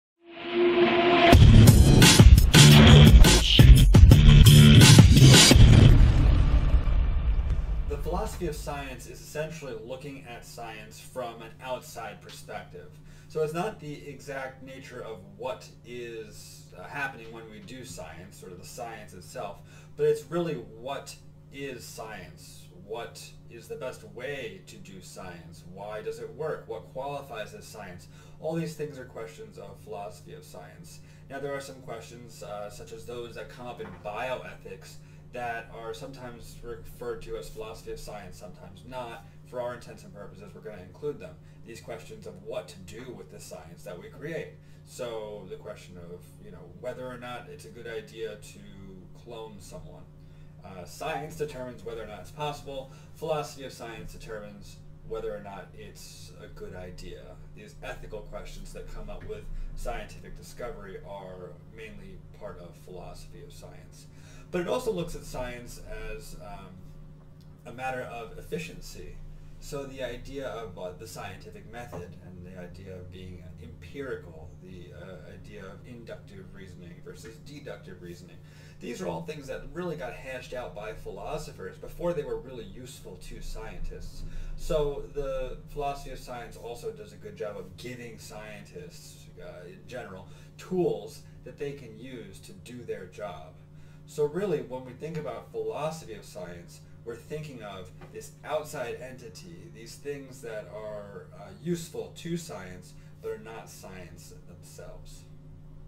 What is the Philosophy of Science?
mattbwrites.com What does philosophy have to do with science?

Quite a bit actually! While scientists deal this things that can be observed, something else needs to cover the things that can't be! Philosophy of science covers questions like
What qualifies as science?
How can we apply scientific discoveries?

Science does have its limitations, and the philosophy of science covers some of those limitations by answering questions that science is unqualified to answer.